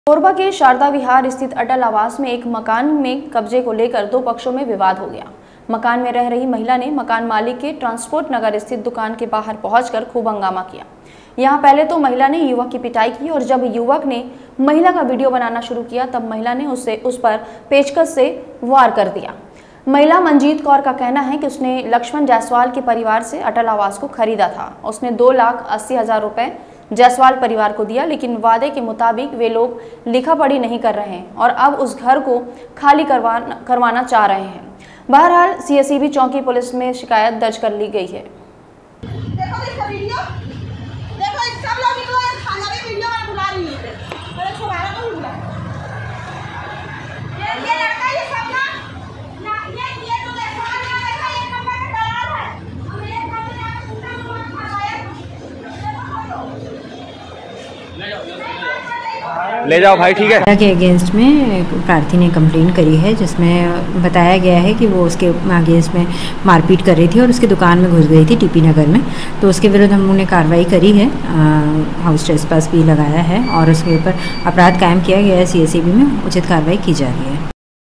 कोरबा : अटल निवास में कब्जे को लेकर हंगामा
Raftaar Media